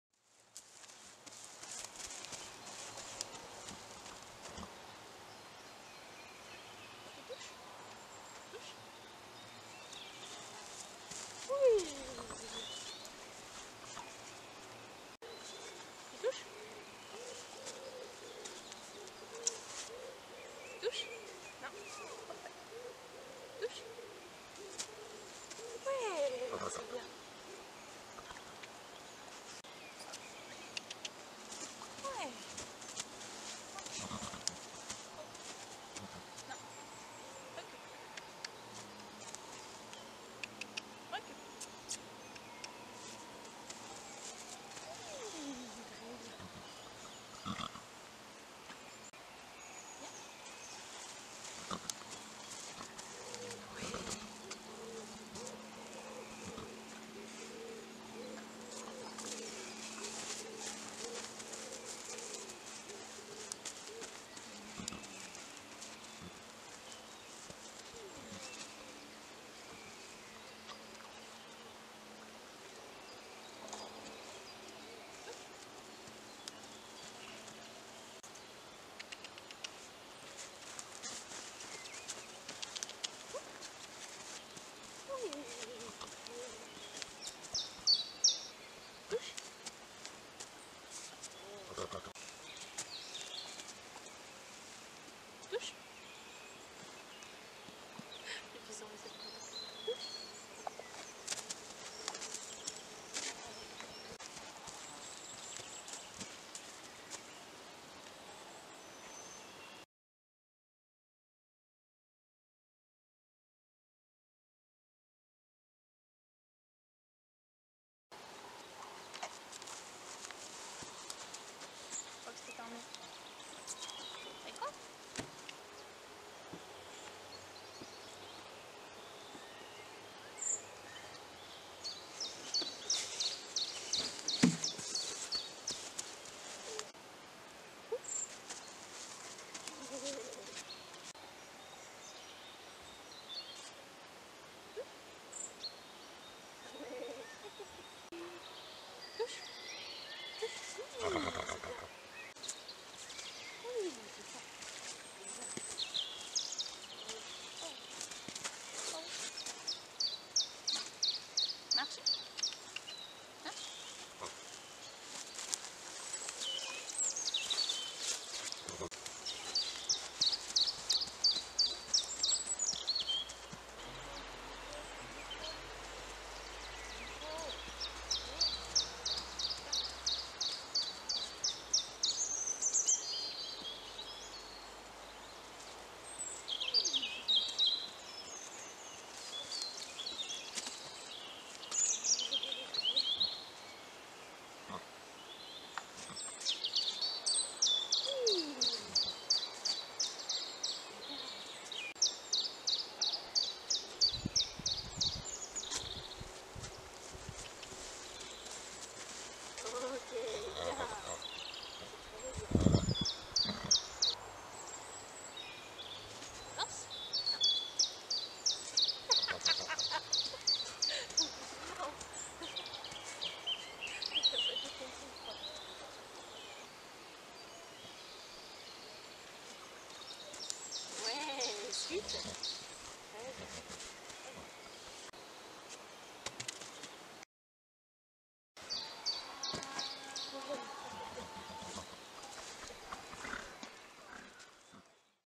TAP et cordelette| Entre R+ et R- , mon cœur balance
Je vous laisse vous renseigner sur le R+ (renforcement positif) et R- (renforcement négatif)

La première et principale chose à savoir c'est que si l'on désire augmenter un comportement, il faut le renforcer, soit en permettant au cheval d'obtenir « quelque chose » d'agréable (c'est ce qu'on appelle renforcement positif) soit en permettant au cheval d'échapper à « quelque chose » de désagréable.

Retrouvez-nous sur Instagram : @epix__studio